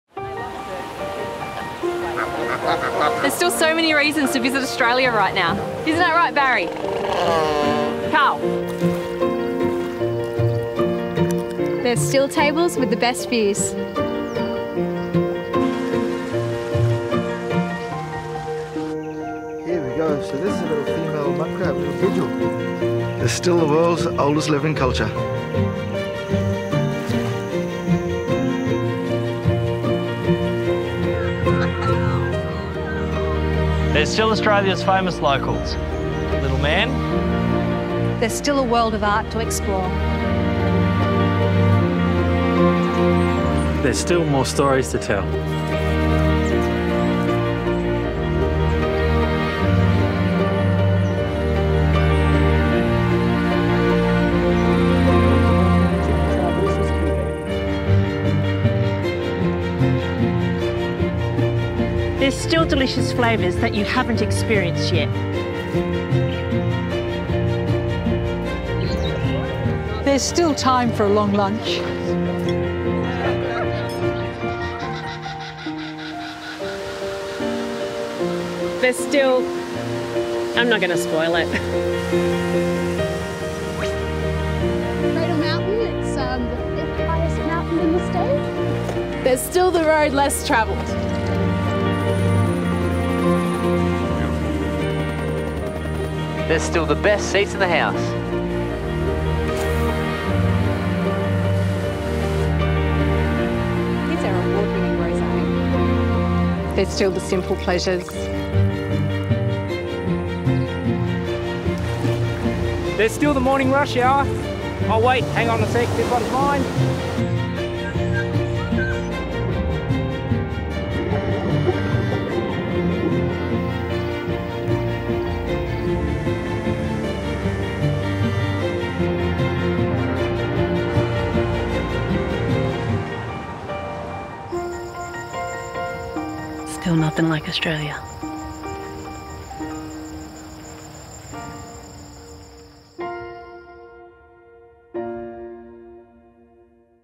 There's Still Nothing Like Australia!
Tourism Australia (TA) is leaning into one of its most successful and longest-standing global campaign platforms, in a push to win back international visitors and rebuild Australia’s reputation following the bushfires and, now, the coronavirus crisis.

The new campaign takes inspiration from TA’s successful There’s Nothing Like Australia global campaign platform, reminding the world that There’s Still Nothing Like Australia.

Spearheading the campaign is this new global broadcast ad, unveiled at Tourism Australia’s Destination Australia Conference on Thursday 12 March in Adelaide.